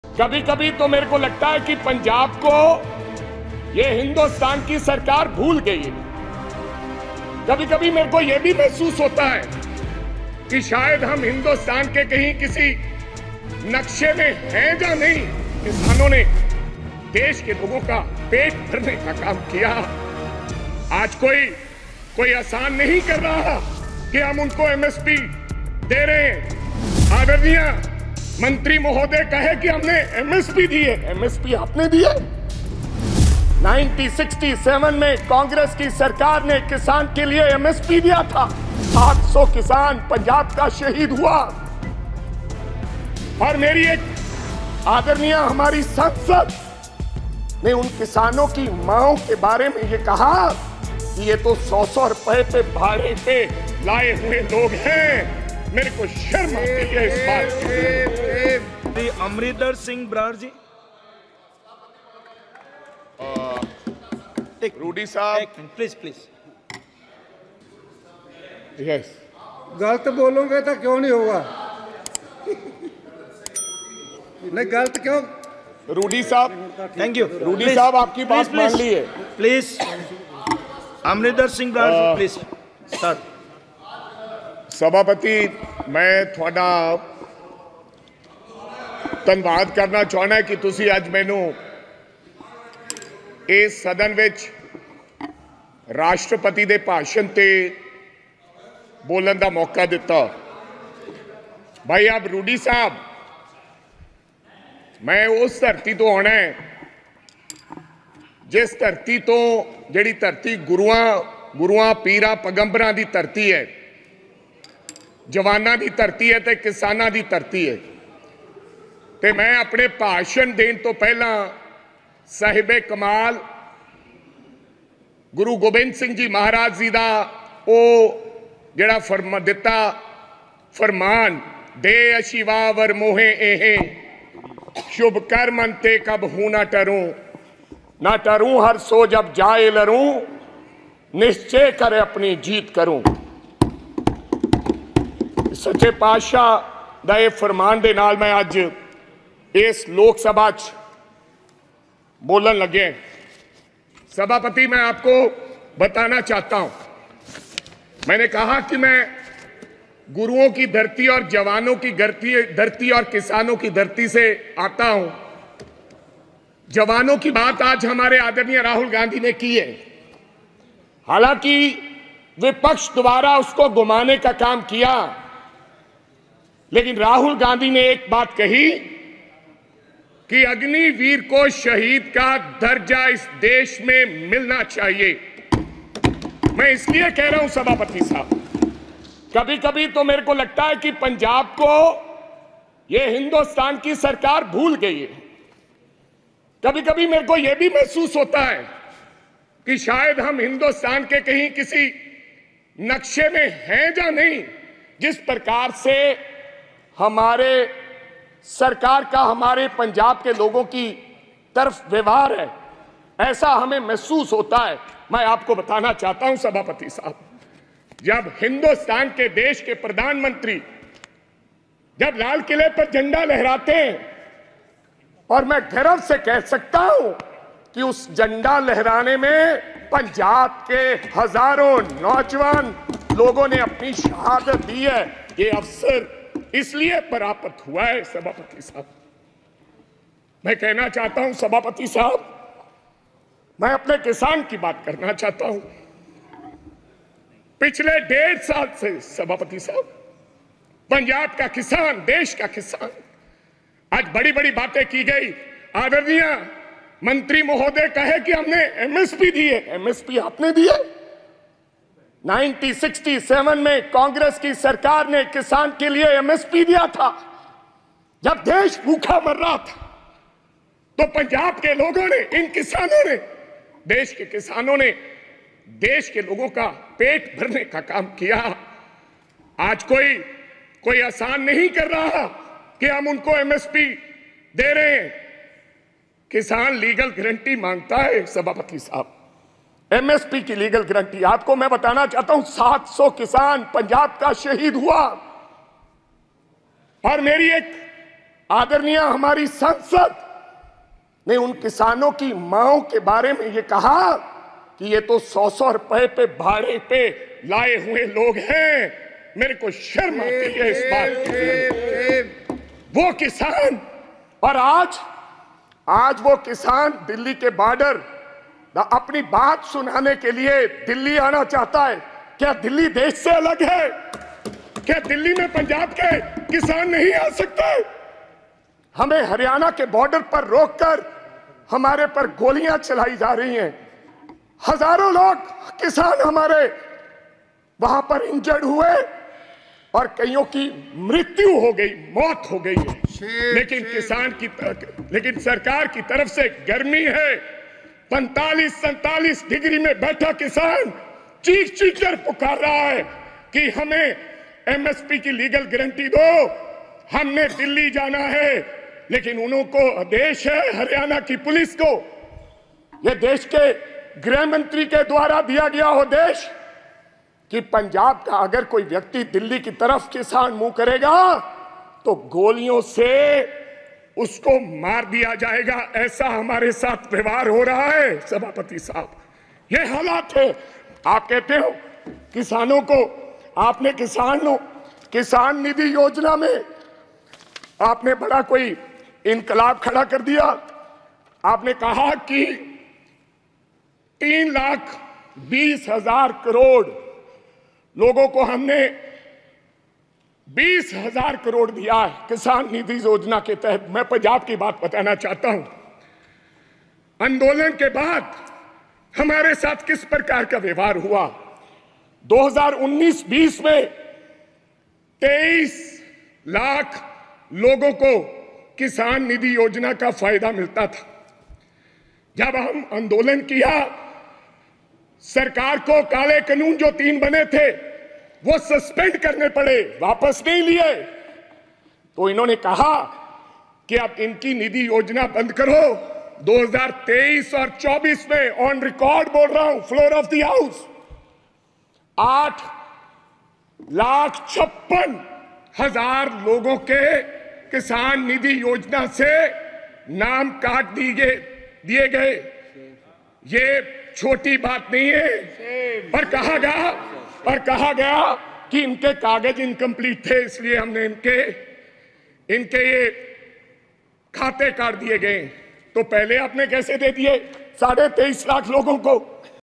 Amrinder Singh Raja Warring on Kangana Ranaut: किसान आंदोलन का जिक्र कर कंगना पर जमकर बरसे |Loksabha
पंजाब के लुधियाना से निर्वाचित व पंजाब कांग्रेस कमेटी के प्रधान अमरिंदर सिंह राजा वड़िंग को संसद में बोलने का जब मौका मिला तो सबसे पहले कंगना पर बरस पड़े  कंगना का नाम लिए बिना अमरिंदर सिंह ने कहा- BJP सांसद ने कहा था- 100-100 रुपये पर भाड़े पर आए है।  सुनिए ये दमदार भाषण, और क्या कहा